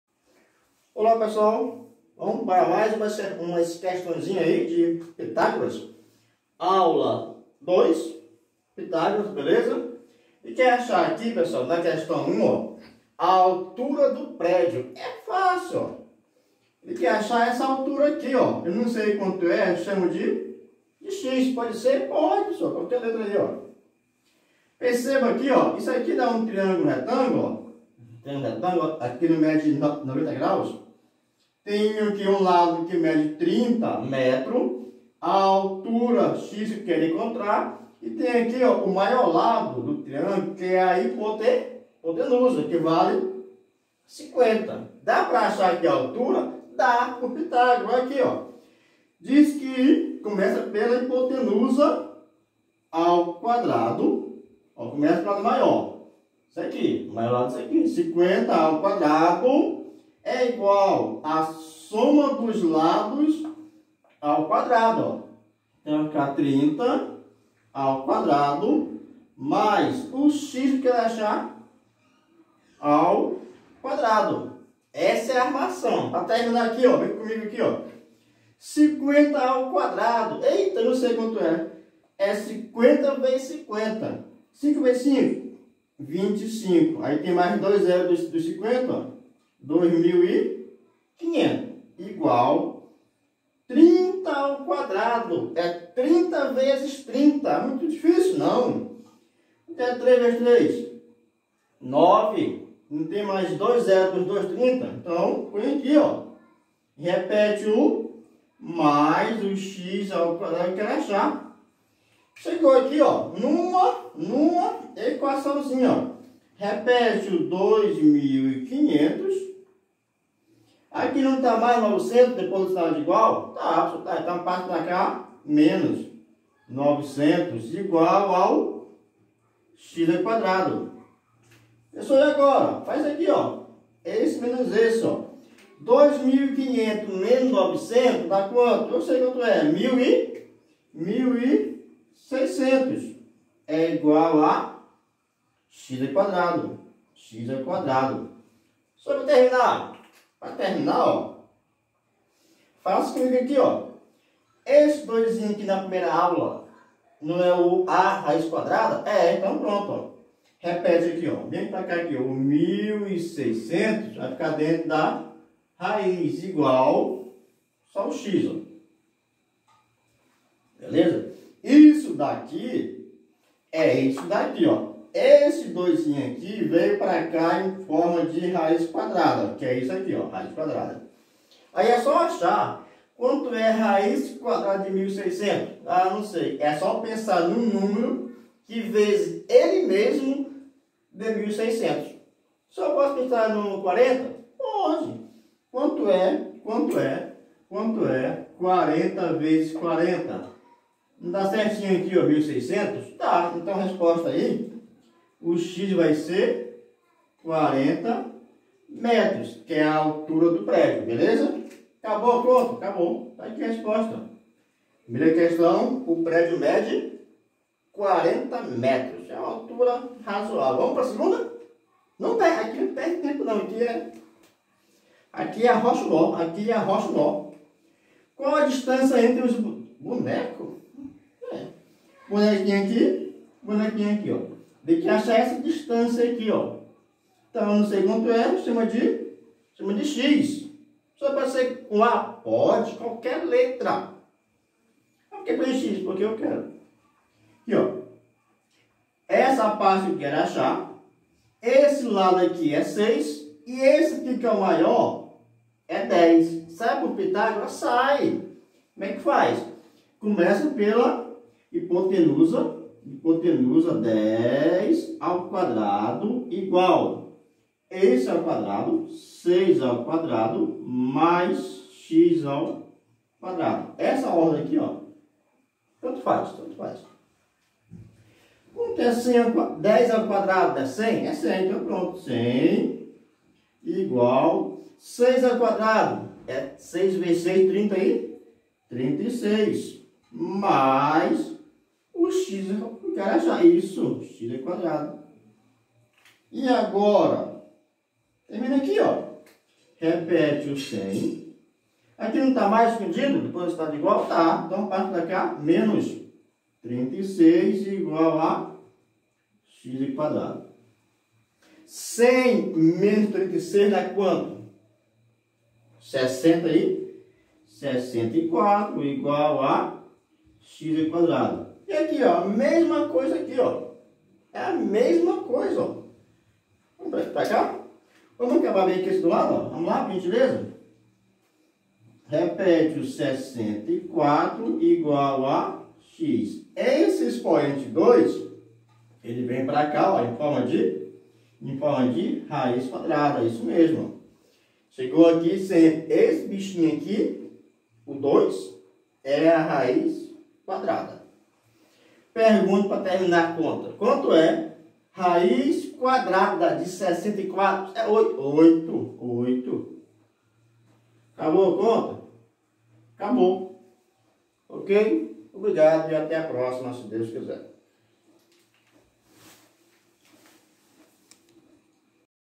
TEOREMA DE PITÁGORAS- AULA 02- EXERCÍCIOS-MATEMÁTICA
Nesta aula aprenderemos a calcular as medidas dos lados do triangulo pelo teorema de Pitágoras aula02